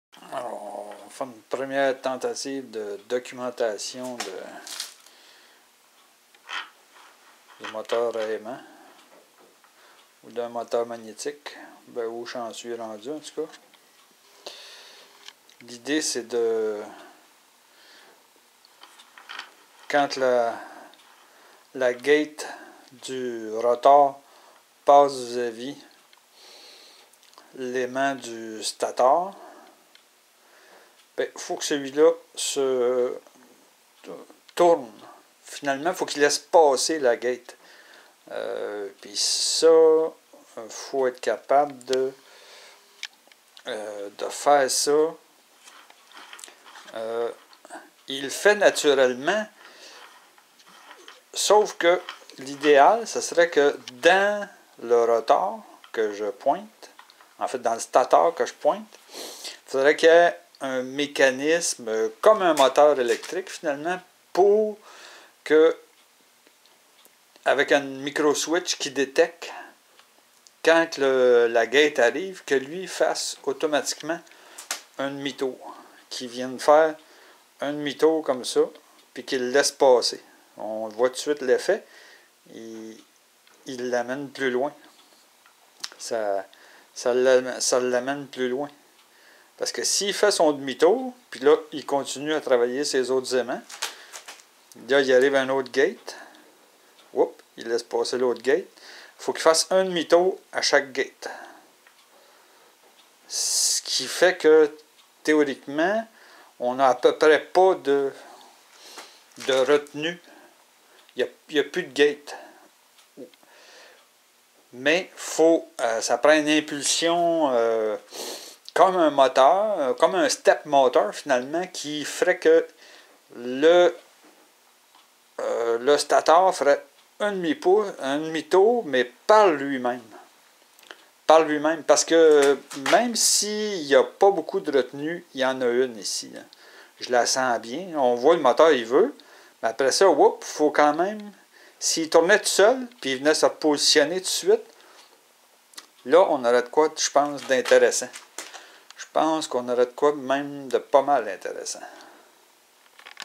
001 - Explication de mon modèle v-gate
Explication du principe de mon moteur v-gate modifié sans gravité puisque à plat.